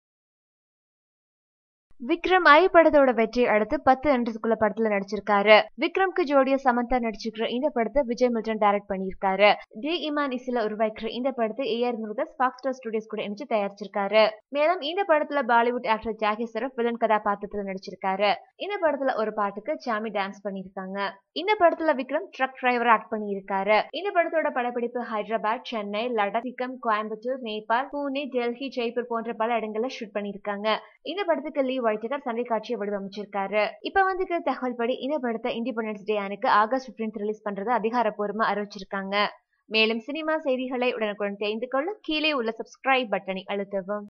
Vikram 10 Enradhukulla Release Date Officially Confirmed -Vijay Milton | Samantha
Vikram 10 Enradhukulla to release on August 15th at an Independence Day